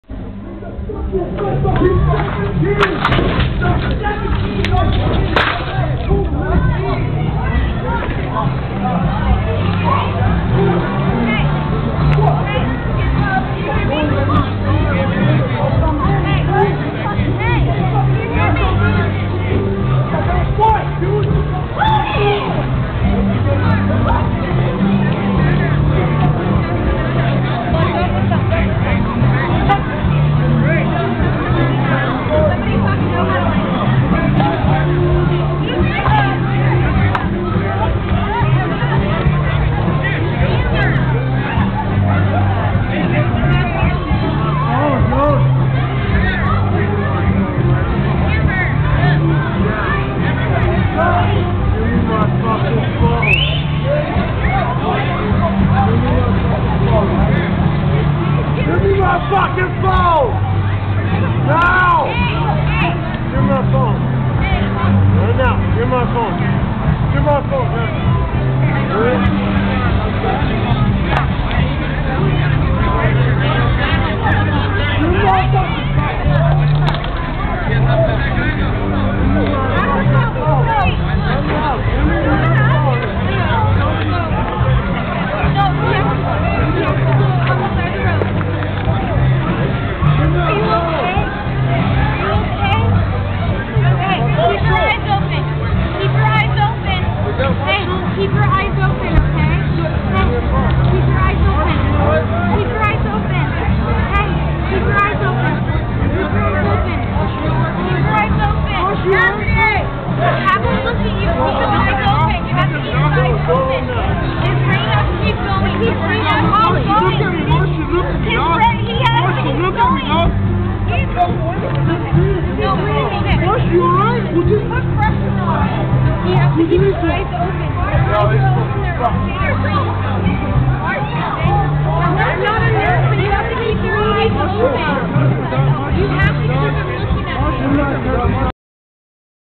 Lakeside Bar fight
A bunch of skinheads crashed the Eastbound bar in Lakeside California looking for trouble and somehow they all woundup
KO in the street.